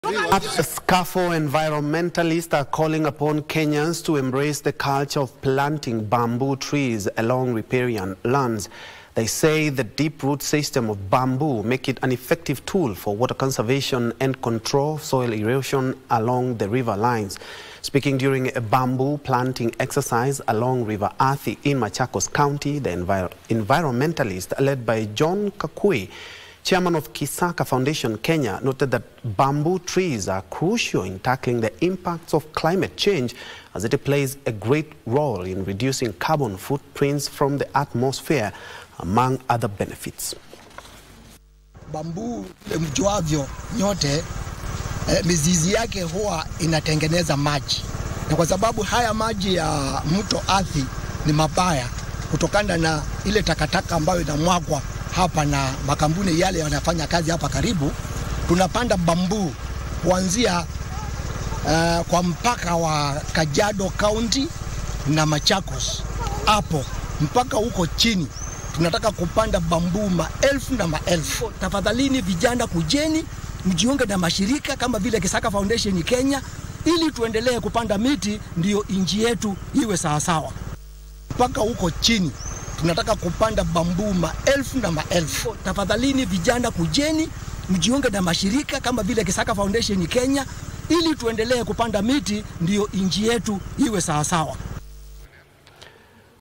Bamboo trees, an effective tool for water conservation along riparian lands
Environmentalists are calling upon Kenyans to embrace the culture of planting bamboo trees along riparian lands. They say the deep root system of bamboo makes it an effective tool for water conservation and control of soil erosion along the river lines.Speaking during a bamboo planting exercise along River Athi in Machakos county, the environmentalists, led by John Kakui, chairman of Kisaka Foundation Kenya, noted that bamboo trees are crucial in tackling the impacts of climate change as it plays a great role in reducing carbon footprints from the atmosphere among other benefits.